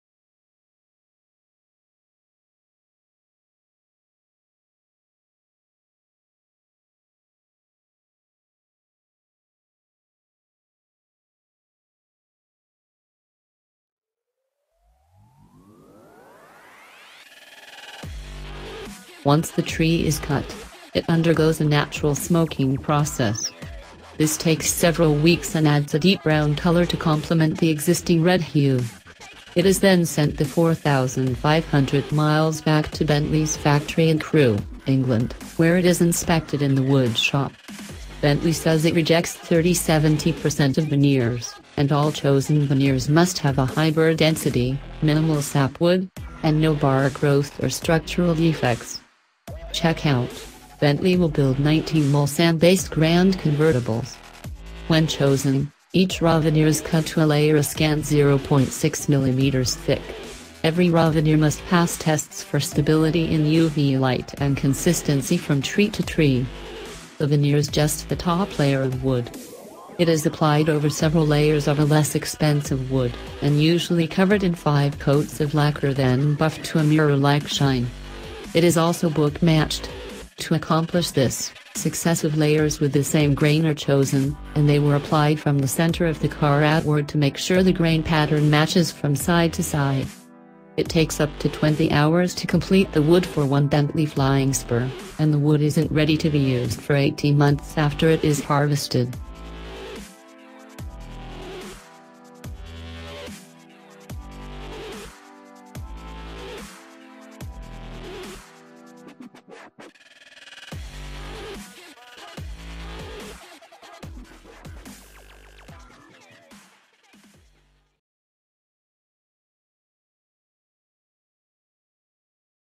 Hot News !!! New Bentley Veneer Hunters Wood Finish
Hot News !!! New Bentley Veneer Hunters Wood Finish - Once the tree is cut, it undergoes a natural smoking process. This takes several weeks and adds a deep brown color to compliment the existing red hue. It is then sent the 4,500 miles back to Bentley's factory in Crewe, England, where it is inspected in the wood shop. Bentley says it rejects 30-70 percent of veneers, and all chosen veneers must have a high-burr density, minimal sapwood, and no bark growth or structural defects.

CHECK OUT: Bentley will build 19 Mulsanne-based Grand Convertibles

When chosen, each raw veneer is cut to a layer a scant 0.6 mm thick. Every raw veneer must pass tests for stability in UV light and consistency from tree to tree.

The veneer is just the top layer of wood. It is applied over several layers of a less expensive wood, and usually covered in five coats of lacquer then buffed to a mirror-like shine. It is also book matched. To accomplish this, successive layers with the same grain are chosen, and they are applied from the center of the car outward to make sure the grain pattern matches from side to side. It takes up to 20 hours to complete the wood for one Bentley Flying Spur, and the wood isn't ready to be used for 18 months after it is harvested.

██████ GET DAILY UPDATES! NOW! ██████